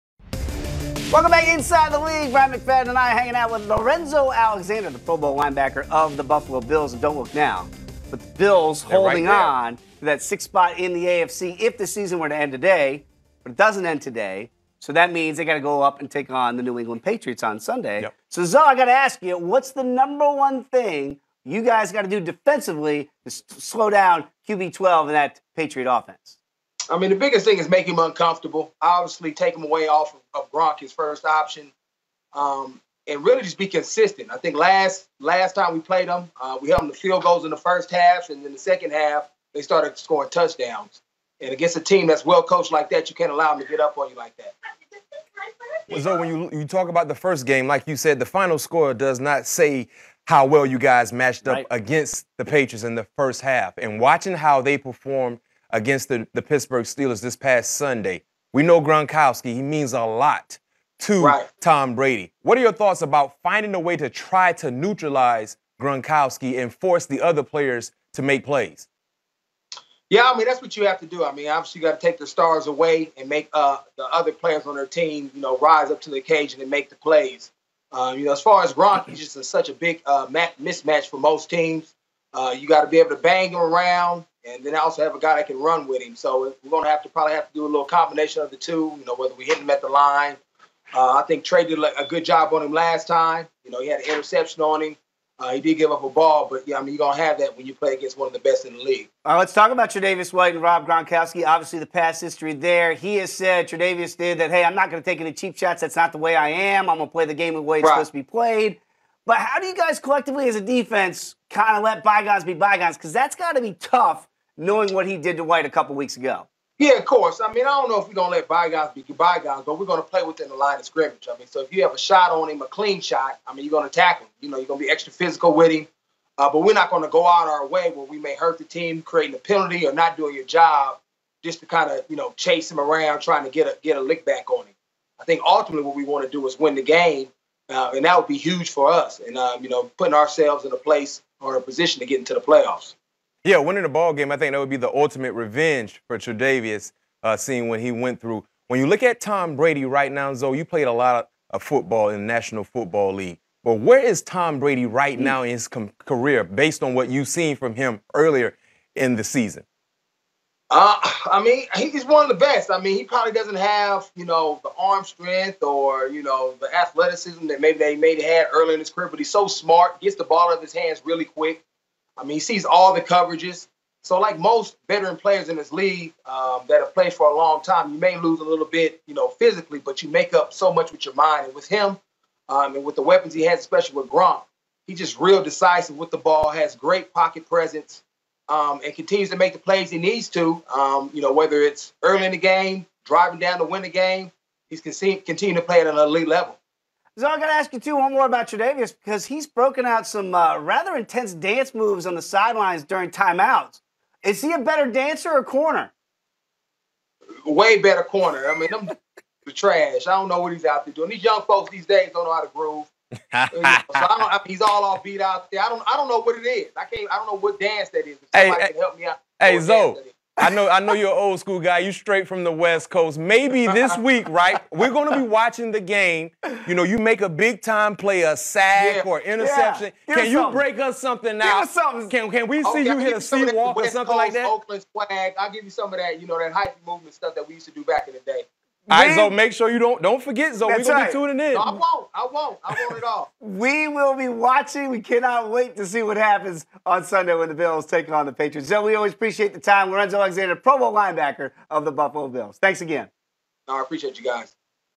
Lorenzo Alexander Previews Bills-Patriots | Stadium
Bills LB Lorenzo Alexander joins the show to discuss Buffalo's trip to face the New England Patriots in Week 16.